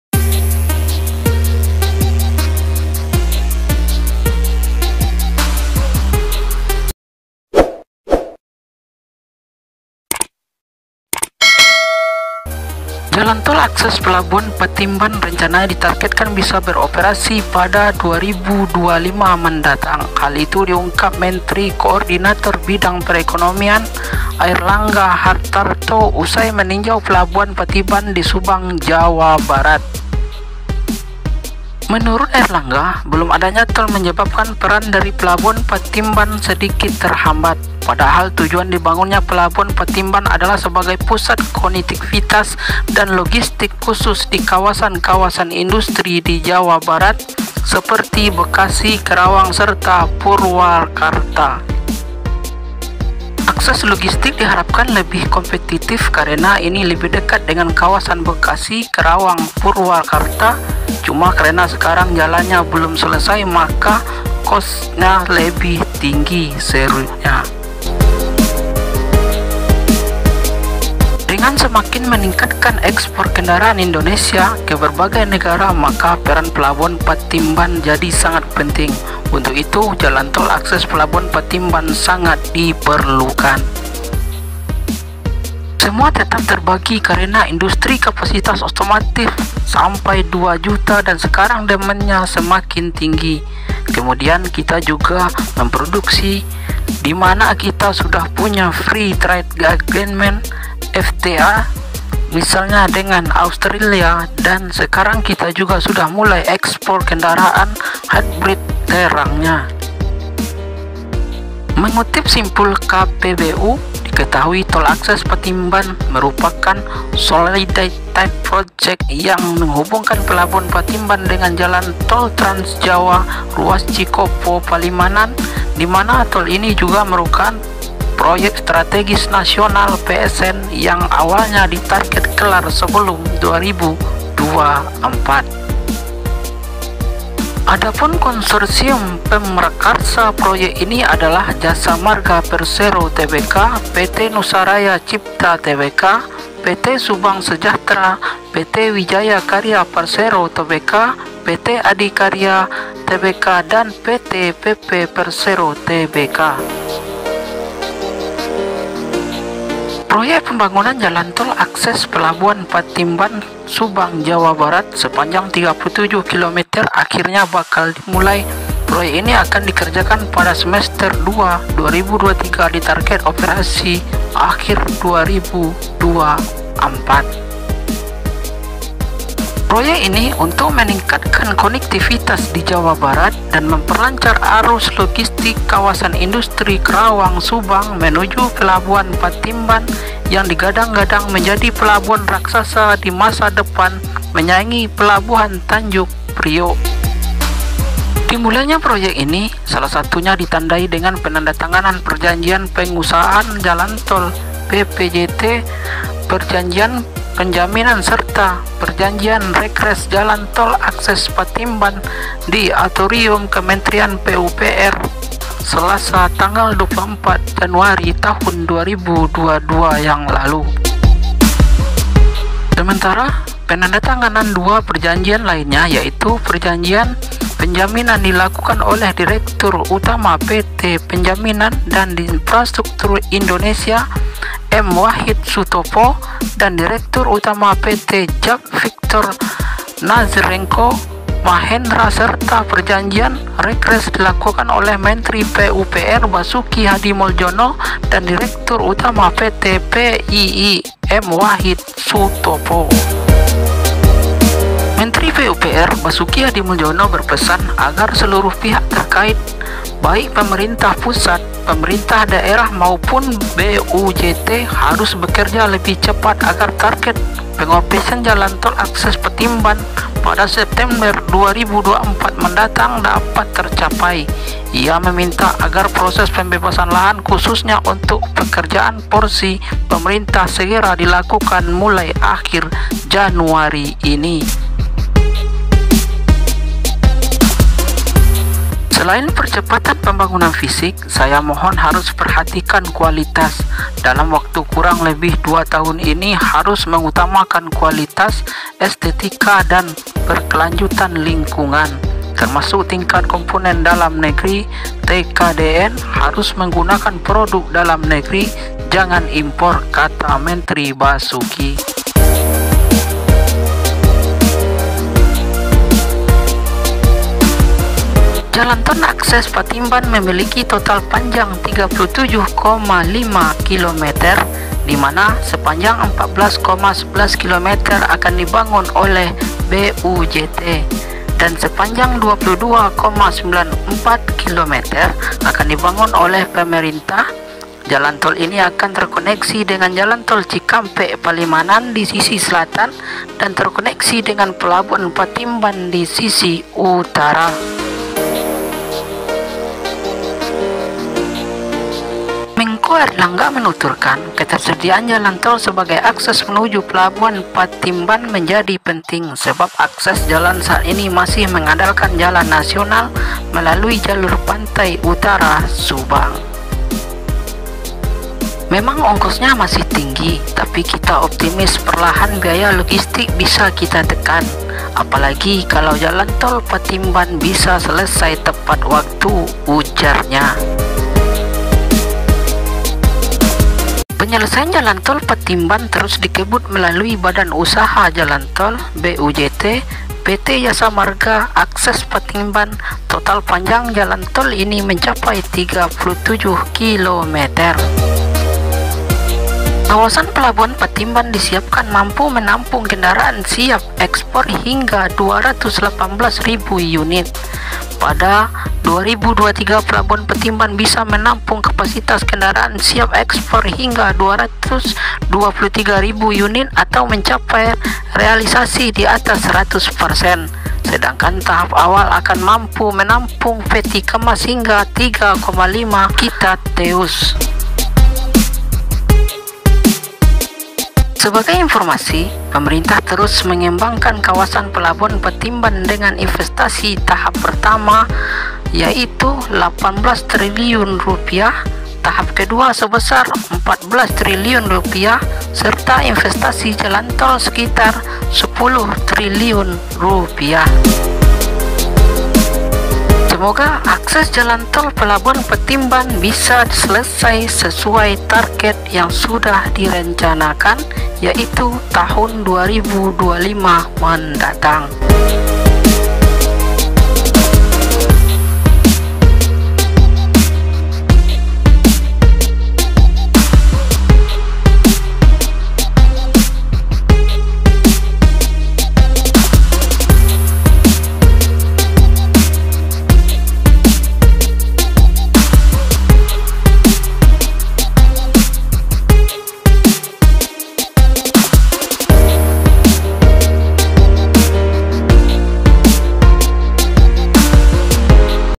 AKSES JALAN TOL PELABUHAN PATIMBAN ❗BERES 2025 MENDATANG
Pembangunan jalan tol akses pelabuhan patimban
Terus dilakukan dan di upayakan pengerjaannya,
Di harapkan pada tahun 2025 sudah bisa di
Manfaatkan dan di operasikan hal itu di sampaikan langsung oleh menteri perekonomian
Bapak Airlangga Hartarto, saat kunjungan kerjanya ke pelabuhan patimban belum lama ini.